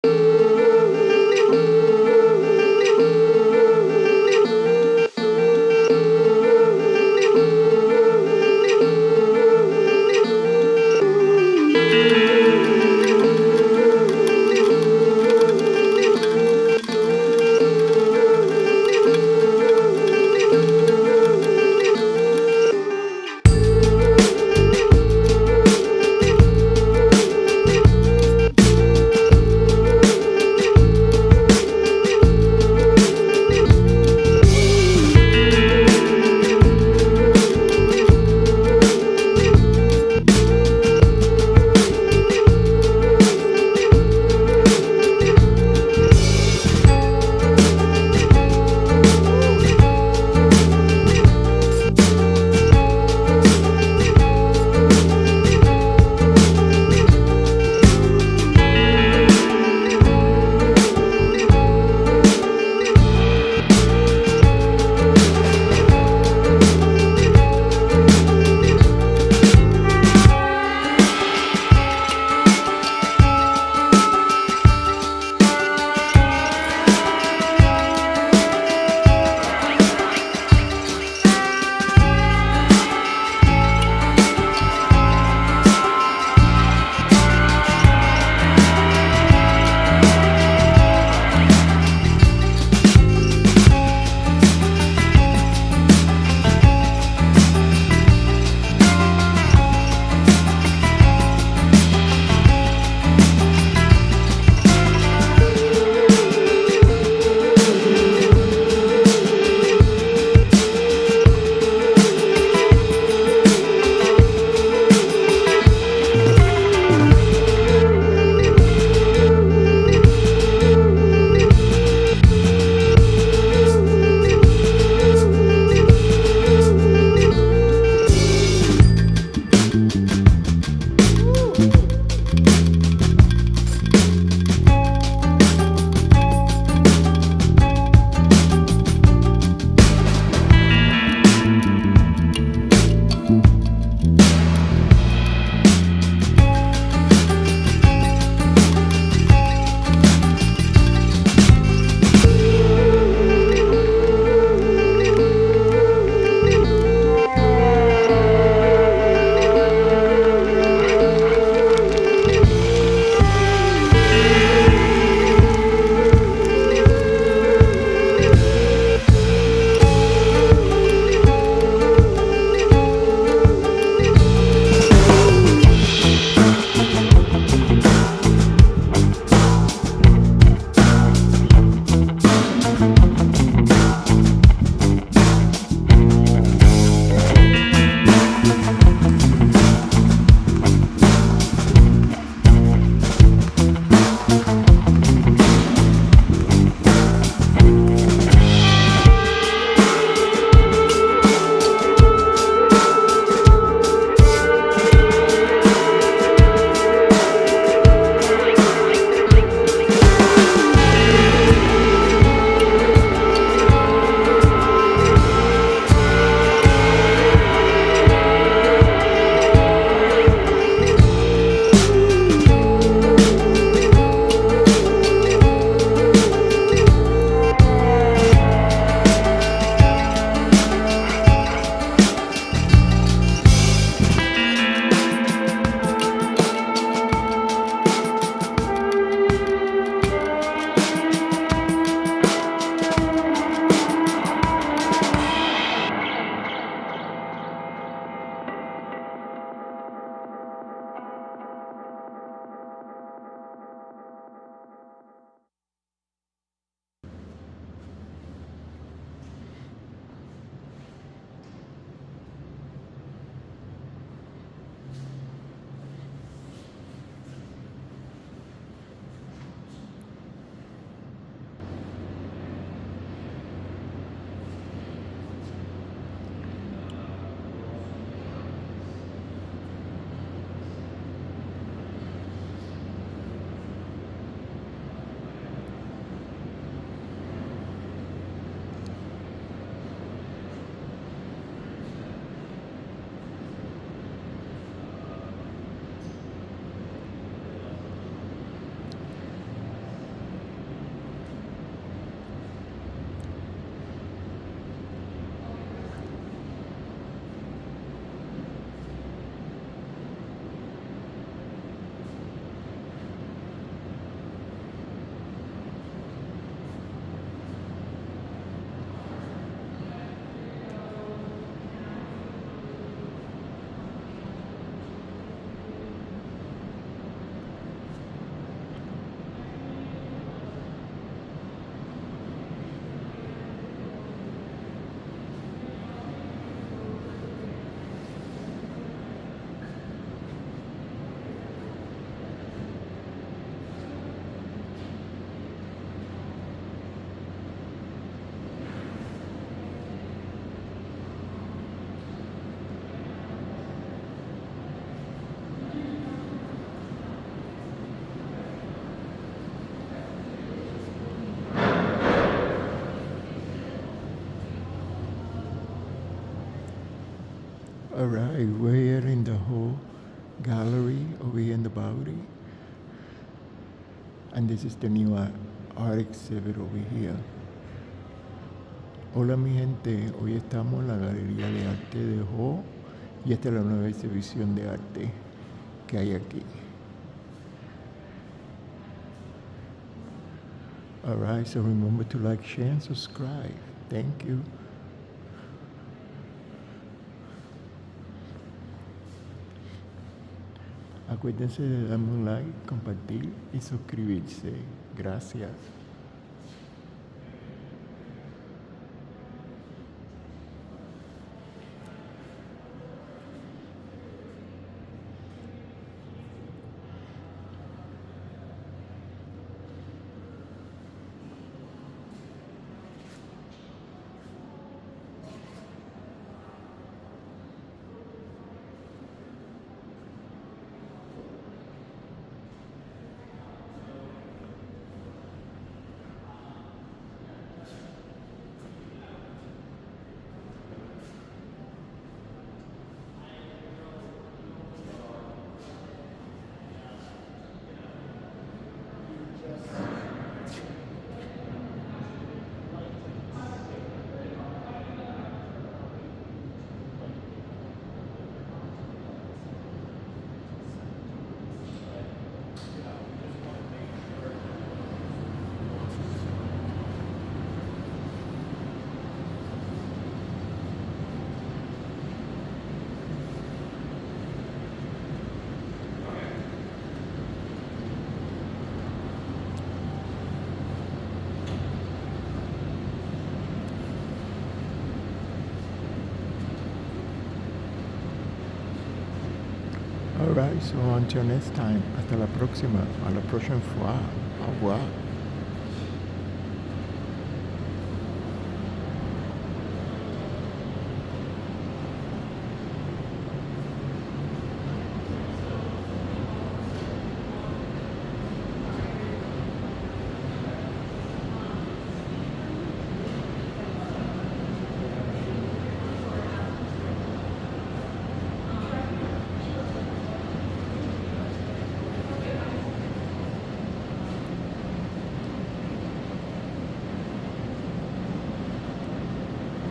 NYC Views An Art Exhibit by artist Andy Dixon at the Hole Bowery Gallery in Noho.
NYC Views An Art Exhibit by artist Andy Dixon called Joy at the Hole Bowery Gallery in Noho.  Una excivision de Arte en El barrio de Noho en la Ciudad de Nueva York. ♥️